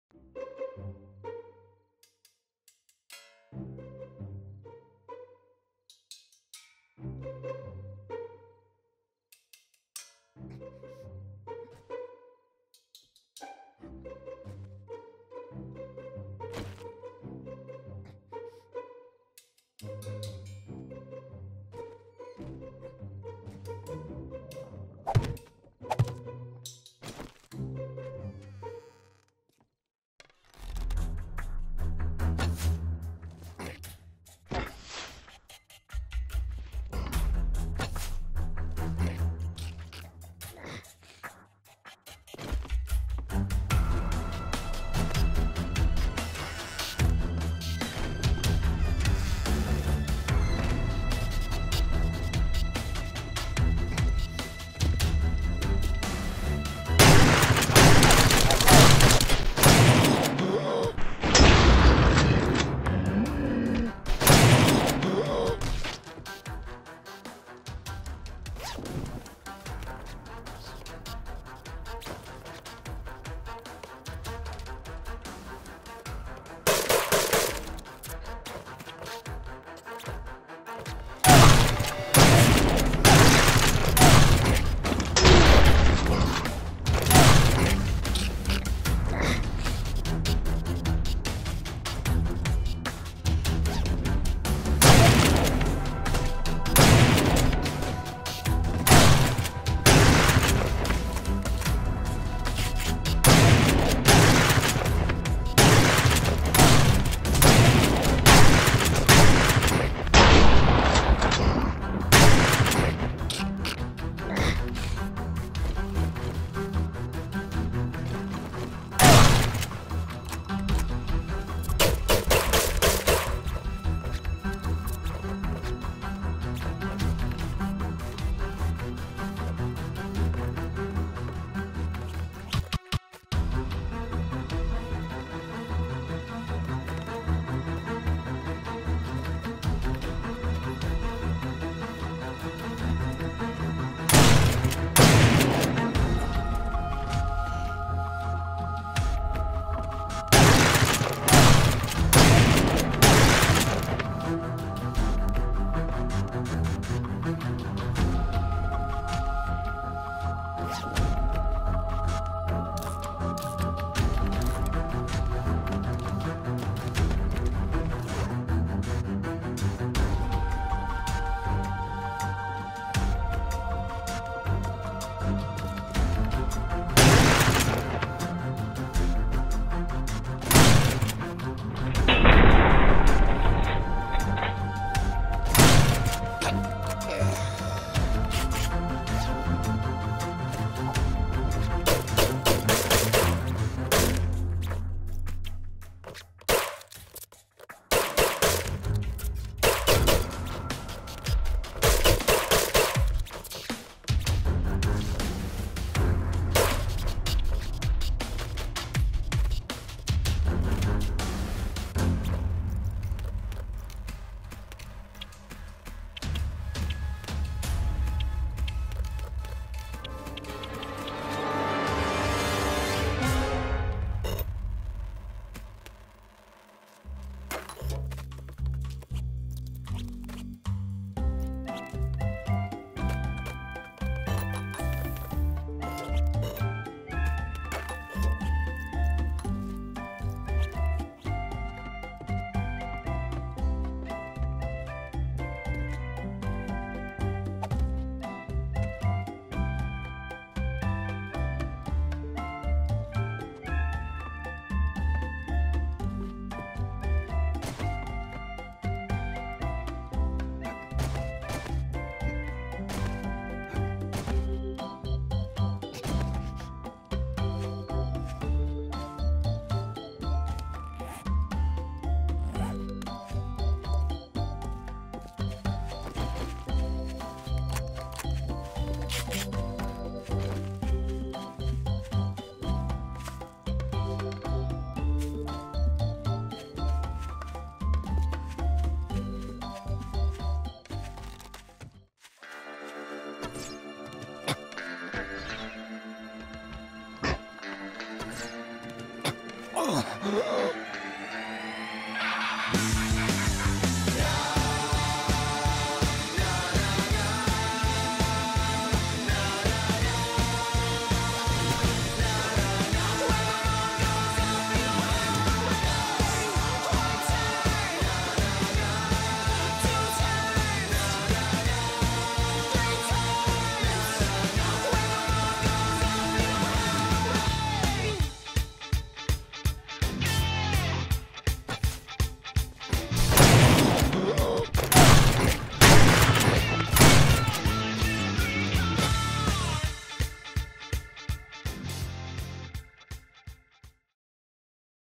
Hold Down Fort
Host is AFK while his buddies hold down fort.